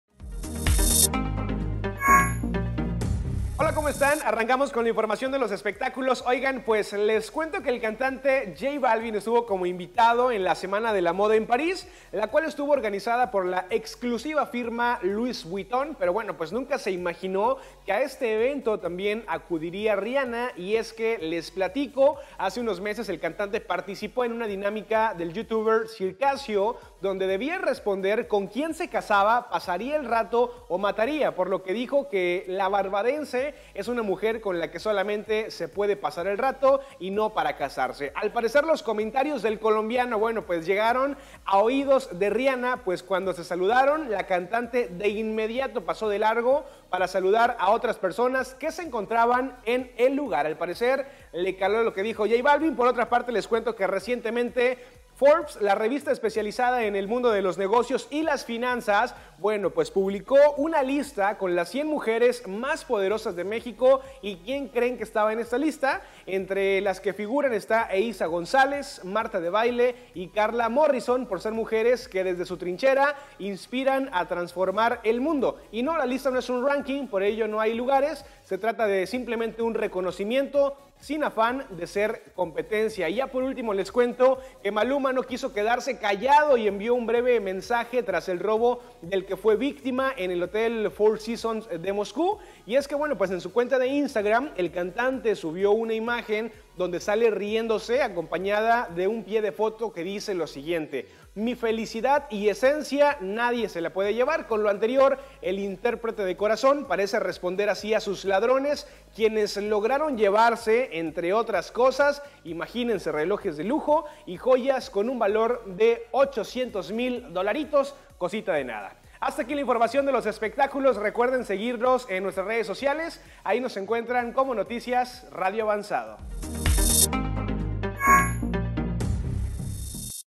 J Balvin aseguró que Rihanna es una mujer solo para pasar el rato
J Balvin se topo con Rihanna después de decir que es una mujer solo para pasar el rato, "Forbes" publicó una lista con las mujeres más poderosas de México y por último, Maluma envió mensaje tras robo en Moscú.